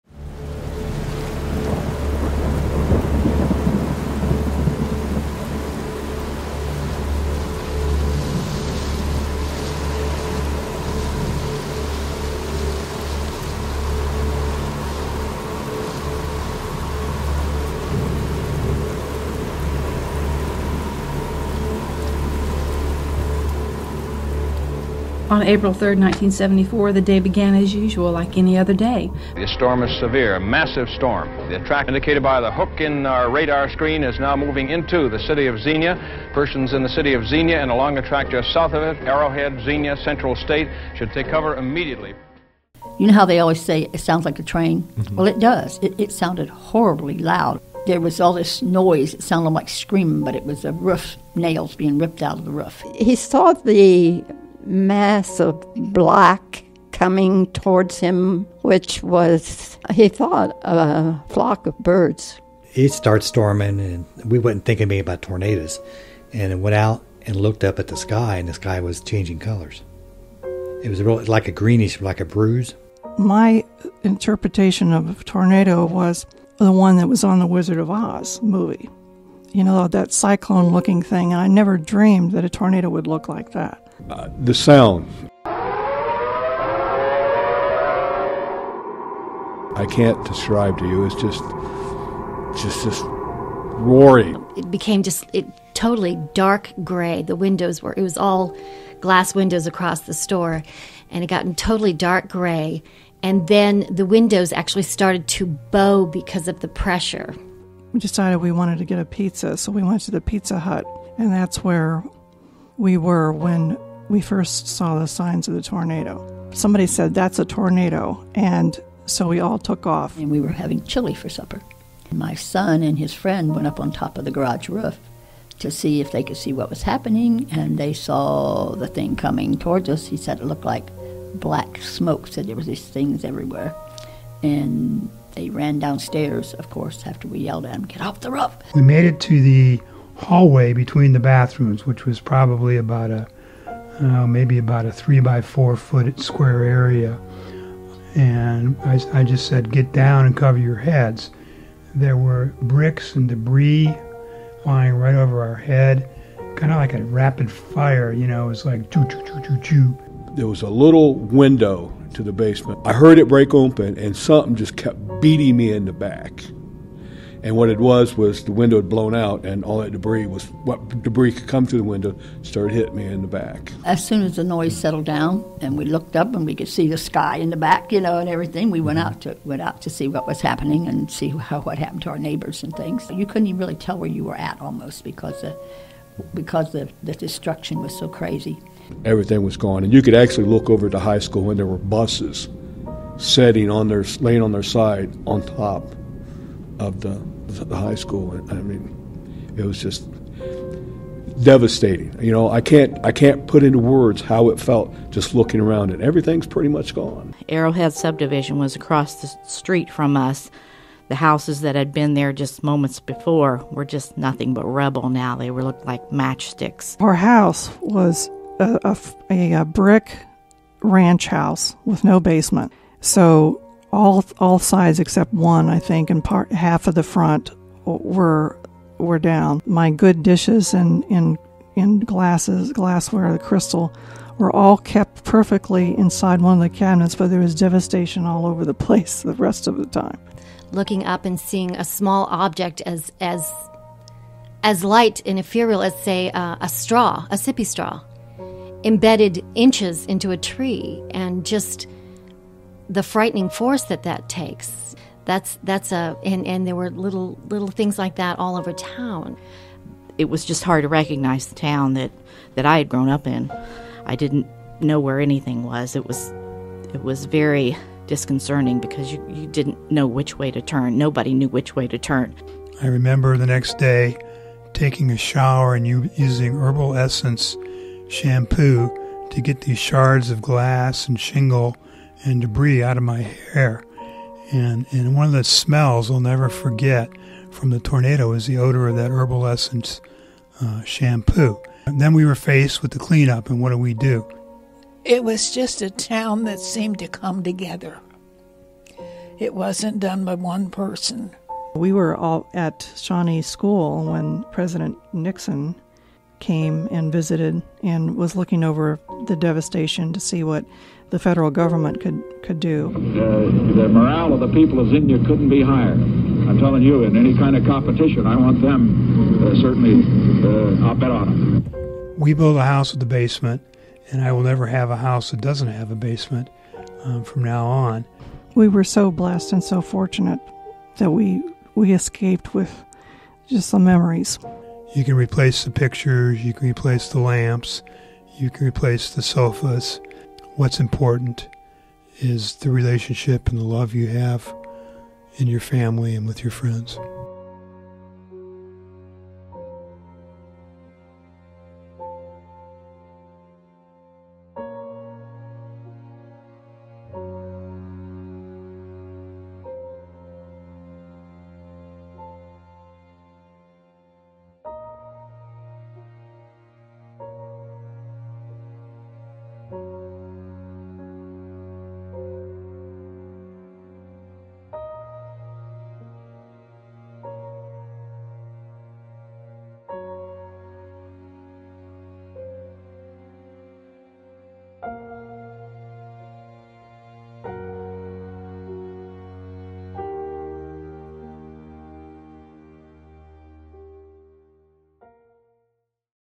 50 Years Later: Remembering the Xenia Tornado
April 3, 2024 marks 50 years since a Super Outbreak of storms across the Midwest. Xenia was one of the cities hit by a devastating F5 tornado, killing 32 and injuring more than 1,000.

Nearly half of the buildings in Xenia were either destroyed or heavily damaged.

These are stories of some Xenians who lived through the tragedy.